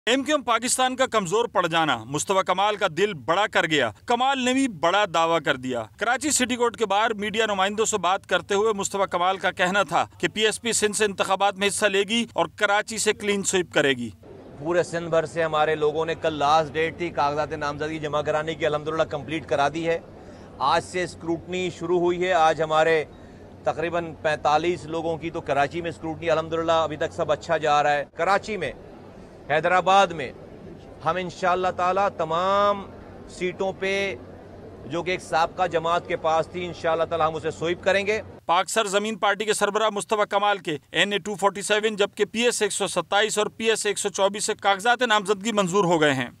پی ایس پی کراچی میں کلین سویپ کرے گی،مصطفیٰ کمال
24 News HD is one of the leading news channels of Pakistan bringing you the latest current affairs from Pakistan and around the world.